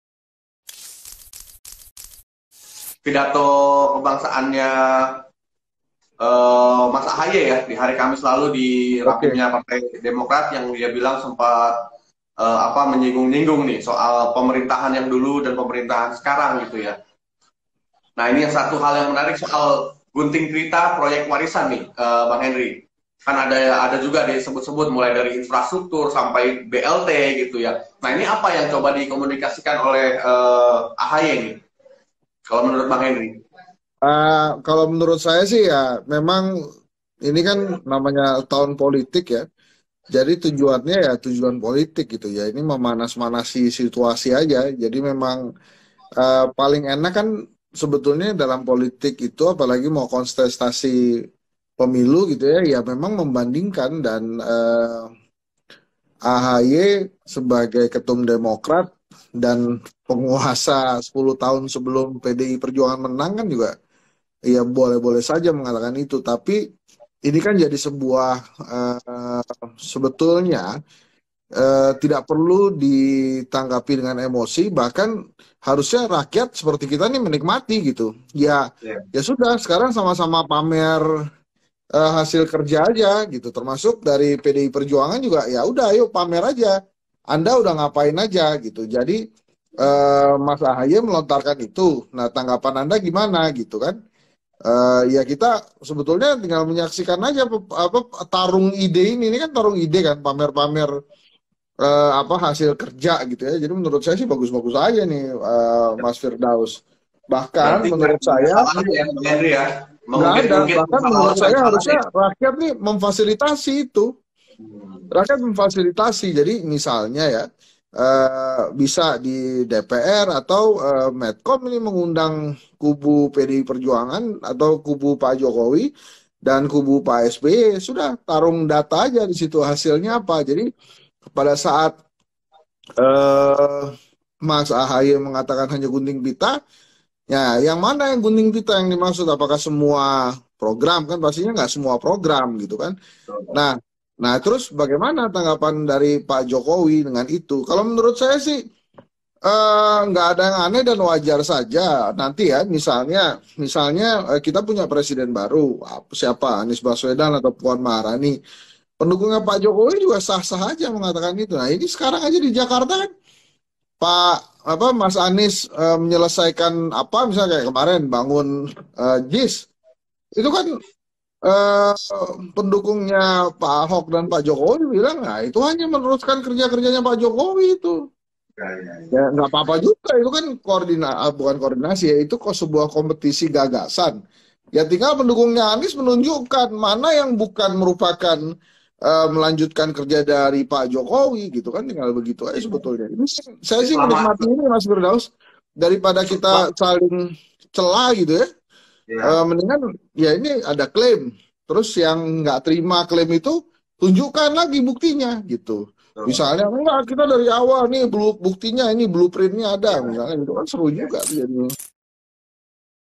Jokowi Disebut Tinggal Gunting Pita, Pengamat: Tak Perlu Ditanggapi dengan Emosi
Ketua Umum Partai Demokrat, Agus Harimurti Yudhoyono (AHY) membuat heboh dengan menyindir Presiden Joko Widodo (Jokowi). Anak Mantan Presiden SBY (Susilo Bambang Yudhoyono) itu menyindir Jokowi hanya modal gunting pita. Menanggapi hal itu, Pengamat Politik Kelompok Diskusi dan Kajian Opini Publik Indonesia (KedaiKOPI), Hendri Satrio menilai pidato kebangsaan AHY memiliki tujuan politik dan memperpanas situasi menjelang Pilpres 2024. Namun hal tersebut tidak perlu diambil serius, sebab masyarakat seharusnya menikmati pertarungan hasil kerja.